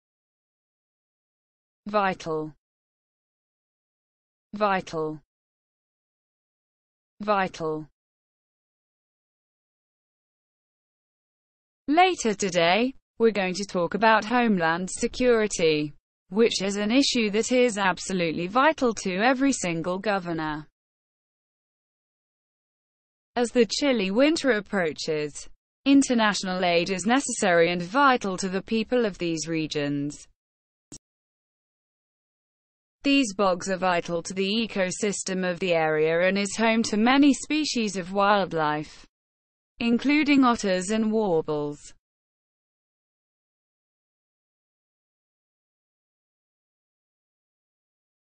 vital: Pronounce vital with Meaning, Phonetic, Synonyms and Sentence Examples
This video shows pronunciation of vital in a sentence, vital meaning, vital definition, vital phonetic, vital synonym and vital example

vital Definition/Meaning : manifesting or characteristic of life
vital Phonetic : 'vaitl
vital Synonyms : essential, indispensable, crucial, key, necessary, needed, required, requisite, important, all-important, of the utmost importance, of great consequence, of the essence, critical, life-and-death, imperative, mandatory, urgent, pressing, burning, compelling, acute, paramount, pre-eminent, high-priority, significant, consequential
vital Examples :
Later today, we're going to talk about homeland security, which is an issue that is absolutely vital to every single governor.
As the chilly winter approaches, international aid is necessary and vital to the people of these regions.
These bogs are vital to the eco system of the area and is home to many species of wildlife including otters and warbles.